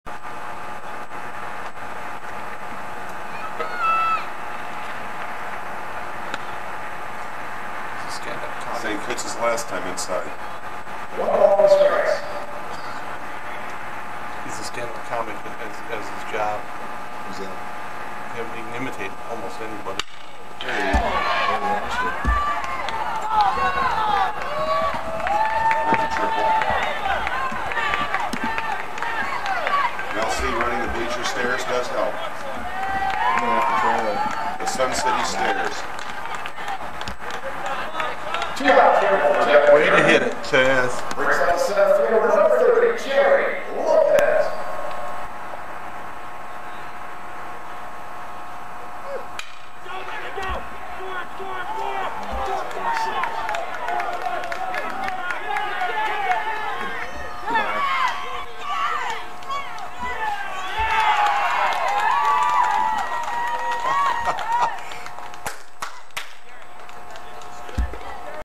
Chaz Helps Rozema cq2cq2
Chaz triples and throws out runner ay the plate to preserve Dave Rozema's  3-1 21 strikeout masterpiece